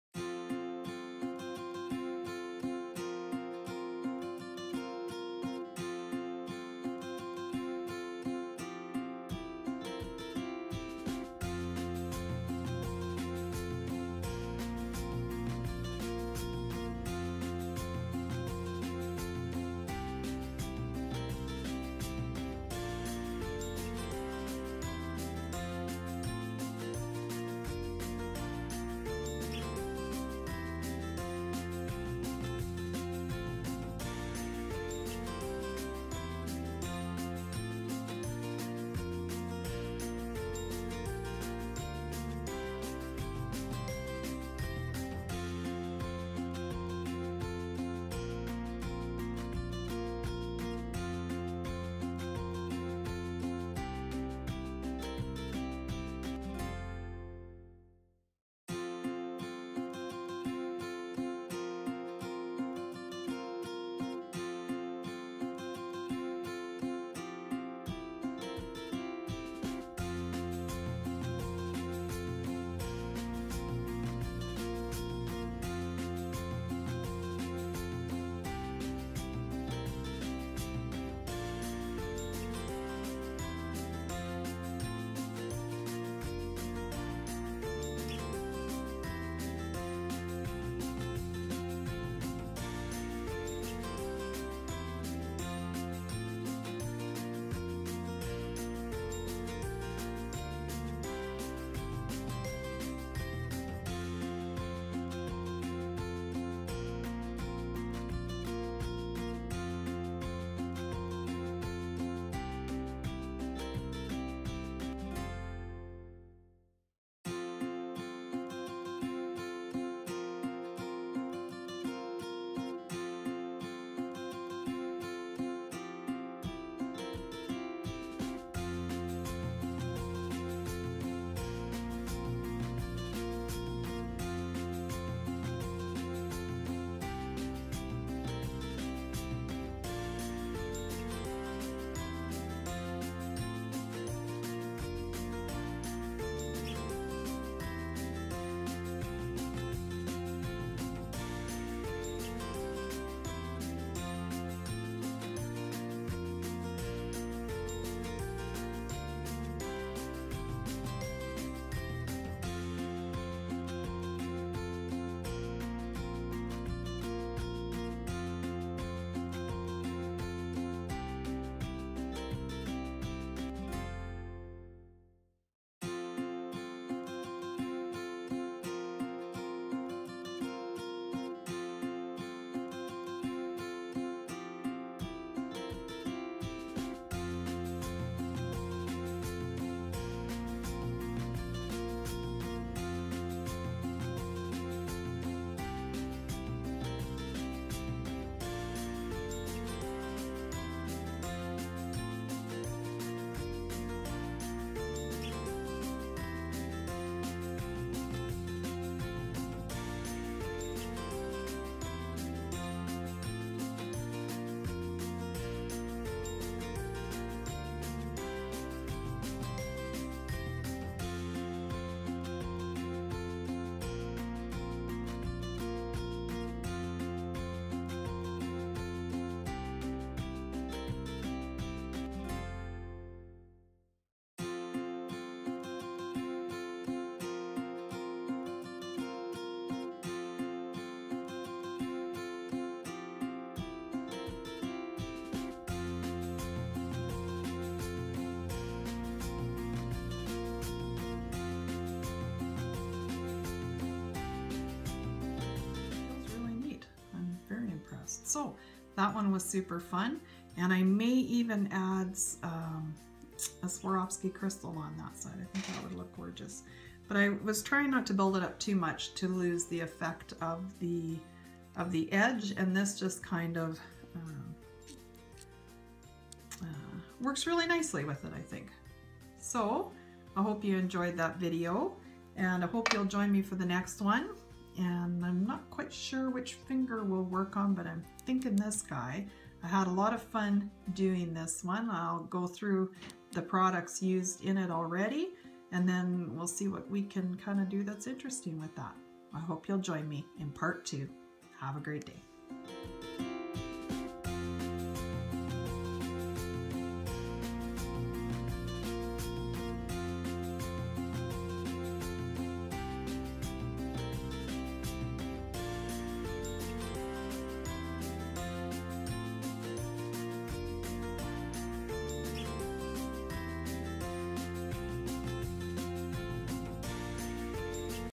Embossed Edge Nail Design- My Nails -Part 1 of 5
Part 1 of a 5 part series of doing my nails with different artwork.
Gels used in video: Amore Ultima